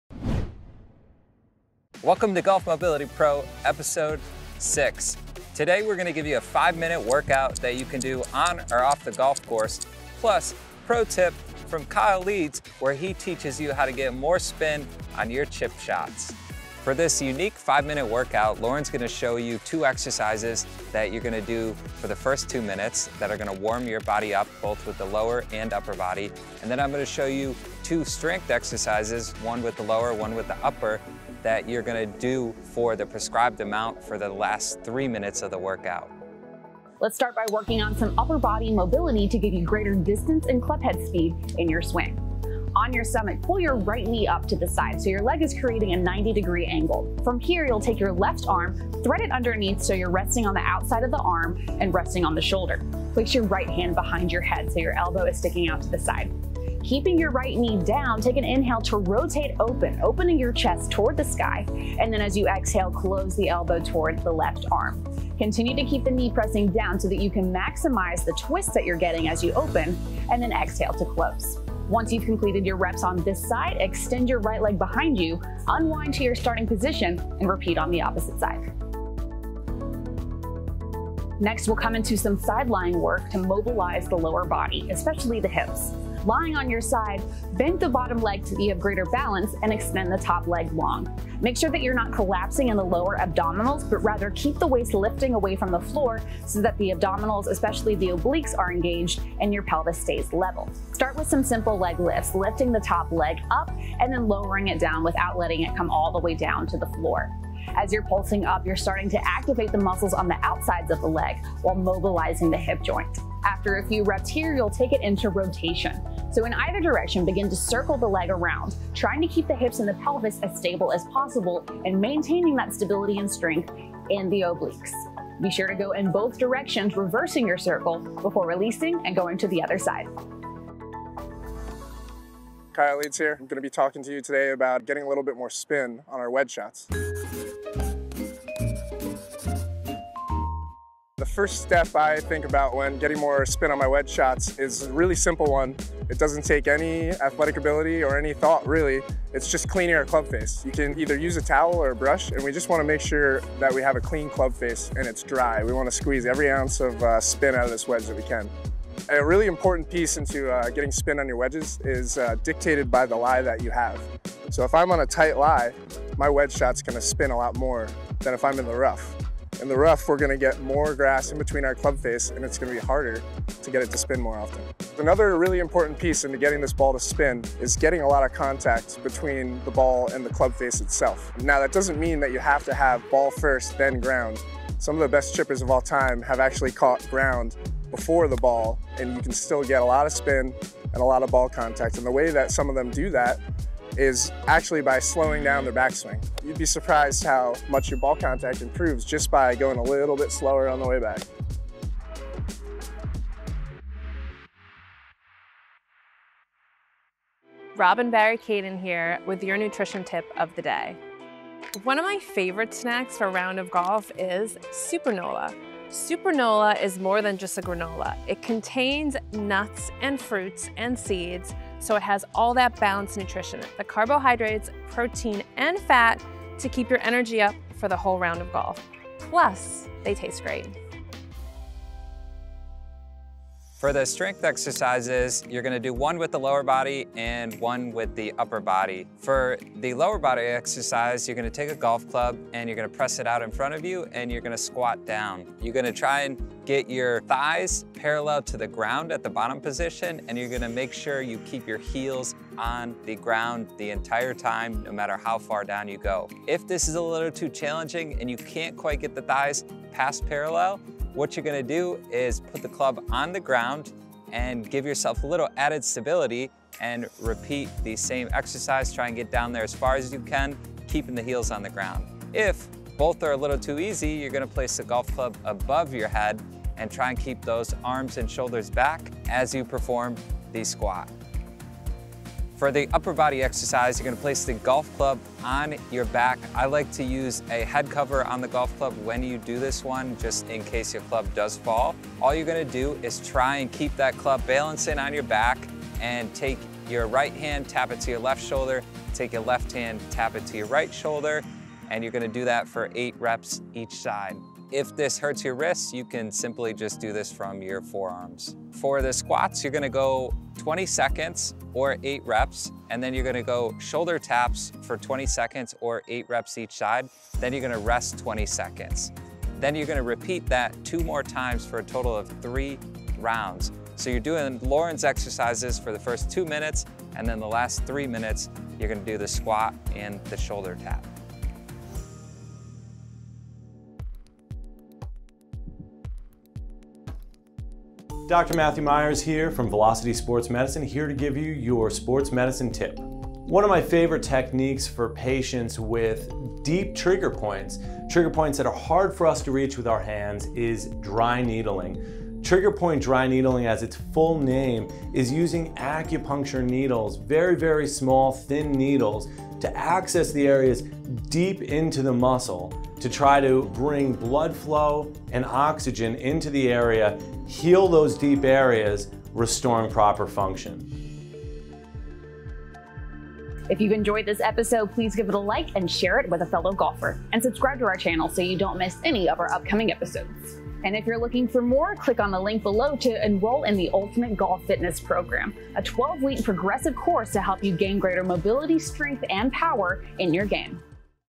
5-Minute Golf Workout (Season 2 - Episode 6)
- Link Below to 12-Week Mobility Program -

Welcome Golfers to Season Two - Episode Six of Golf Mobility Pro!

In this episode, Brandon and Lauren show you a total of 4 exercises to combine for a great 5-minute golf workout!

Up next, professional golfer Kyle Leeds explains how to get more spin on your pitch shots.  Followed by Sports Nutrition Consultant Robin Barrie who shares with you one of her favorite foods for the course!

Finally, Dr. Matt Meyers of Velocity Sports Medicine in Westport, CT discusses the benefits of Dry Needling!

Also, special thanks to our Videographer and Editor Vincent Mizzoni.